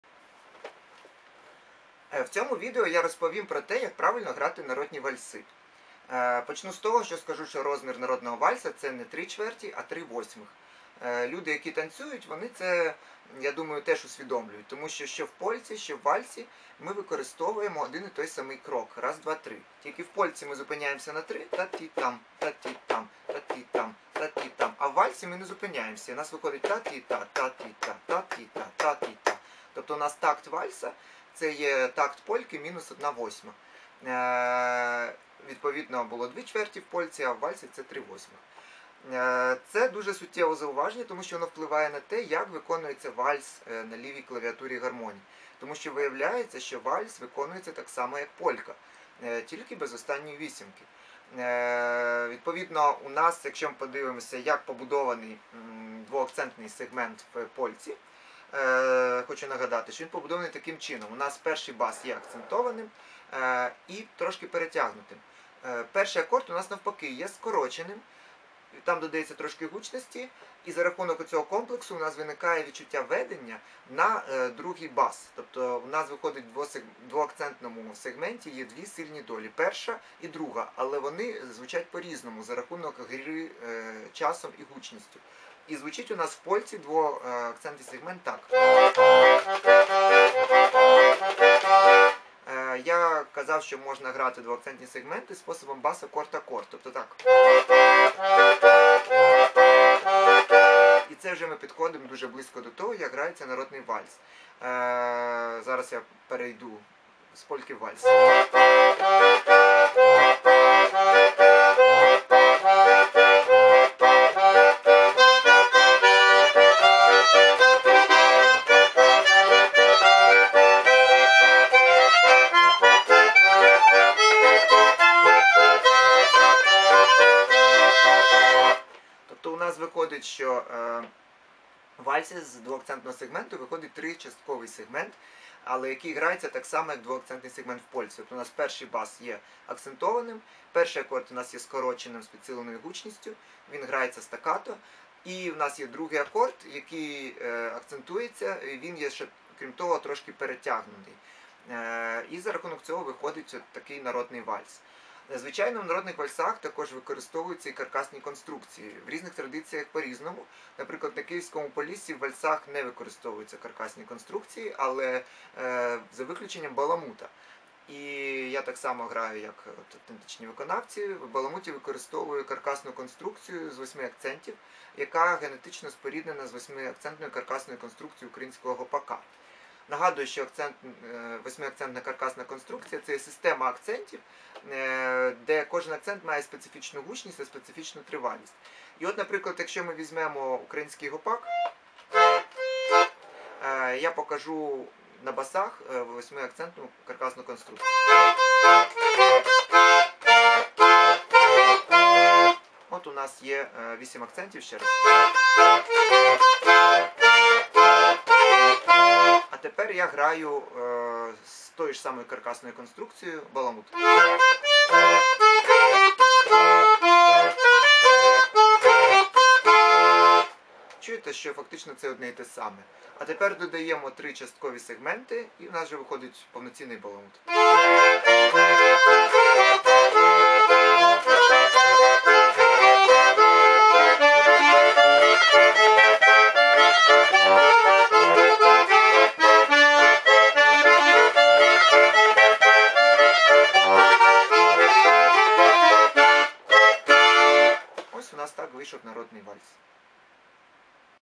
Як зіграти вальс по народному // How to play the waltz on the diatonic accordion
В цій лекції керівник ансамблю "Божичі" Ілля Фетисов розповідає, як правильно грати народні вальси і що спільного між двоакцентними та тричастковими танцями.